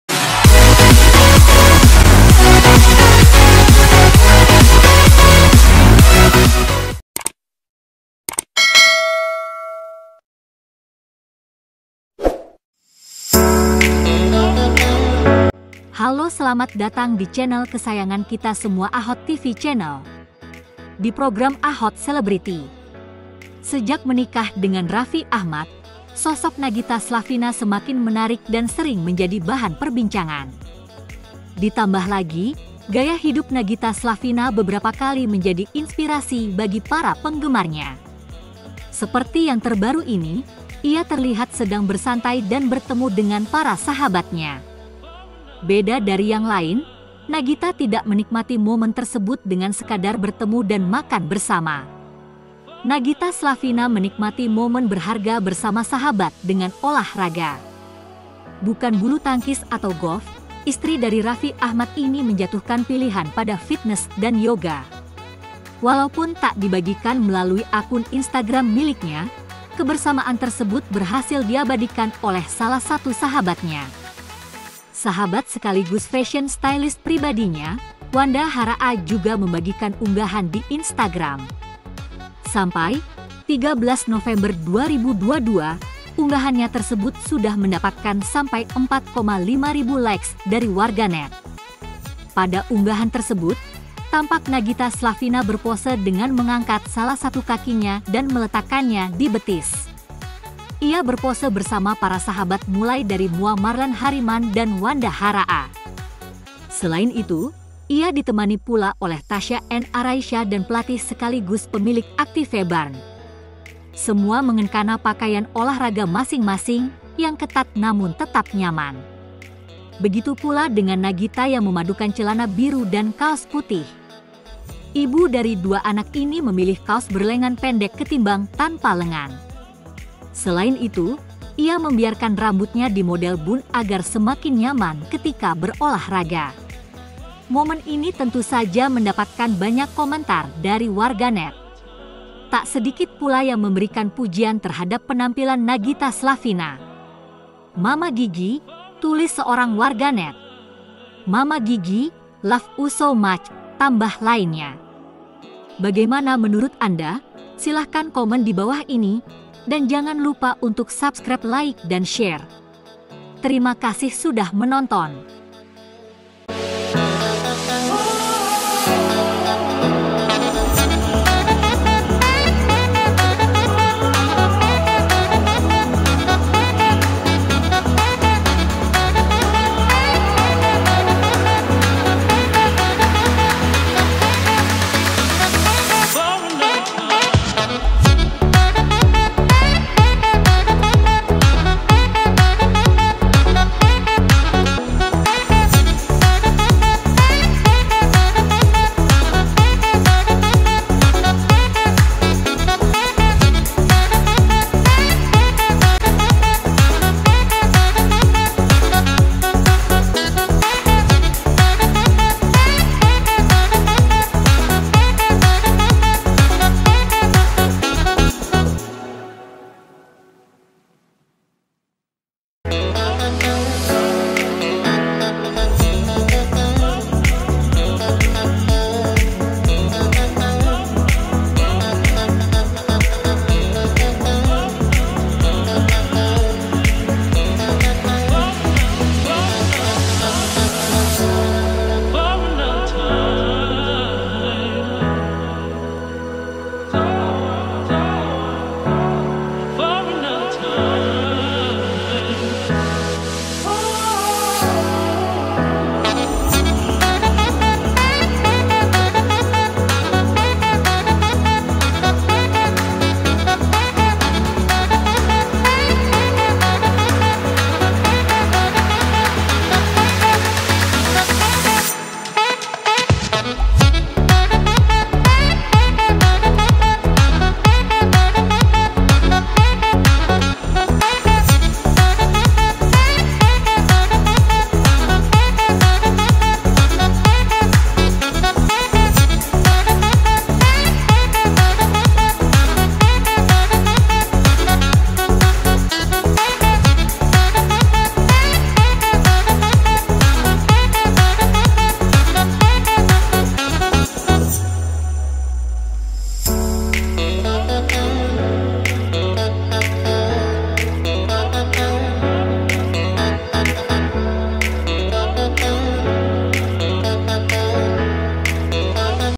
Nagita Hot Info | Nagita Slavina Olahraga Yoga Bareng Sahabat, Penampilannya Banjir Pujian Warganet
Sejak menikah dengan Raffi Ahmad, sosok Nagita Slavina semakin menarik dan sering menjadi bahan perbincangan. Ditambah lagi, gaya hidup Nagita Slavina beberapa kali menjadi inspirasi bagi para penggemarnya.

Seperti yang terbaru ini, ia terlihat sedang bersantai dan bertemu dengan para sahabatnya. Beda dari yang lain, Nagita tidak menikmati momen tersebut dengan sekadar bertemu dan makan bersama.

Nagita Slavina menikmati momen berharga bersama sahabat dengan olahraga.  Bukan bulu tangkis atau golf, istri dari Raffi Ahmad ini menjatuhkan pilihan pada fitness dan yoga.